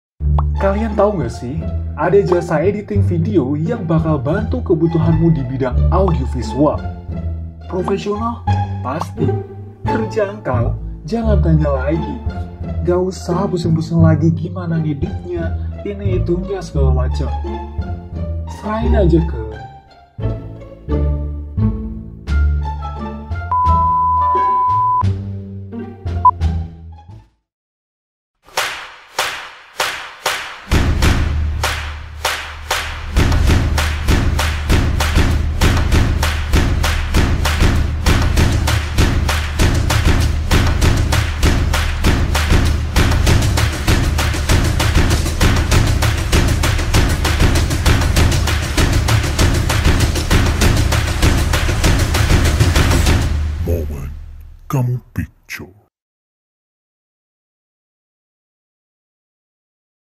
Iklan Online Momen Kamu Picture MKProduksi Periklanan2021
Nama                   : Wahyu Eko Fitriyanto
NIM                      : J3A119240
Kelas                   : A - 1
Dosen Pengajar MK Produksi Periklanan (Praktikum)  : Mulyono S.Pt., MTI
Dosen Pengajar MK Produksi Periklanan (Kuliah)         : Muhammad Al Farissy, S.I.Kom., M.Sc
Program Studi Komunikasi Sekolah Vokasi IPB

Produk   : Momen Kamu Picture
   Momen Kamu Picture merupakan sebuah jasa yang bergerak di bidang editing video (audio visual). Jasa ini menawarkan bantuan kepada konsumen yang berkaitan dengan editing audio visual. Momen Kamu Picture melayani berbagai jenis editing audio visual seperti video iklan produk, company profile, Wedding, Graduation, Cinematic Video, dan lain-lain.

DESCLAIMER
[IKLAN INI DIBUAT DAN DIPRODUKSI DALAM RANGKA MEMENUHI TUGAS DARI MATA KULIAH PRODUKSI PERIKLANAN]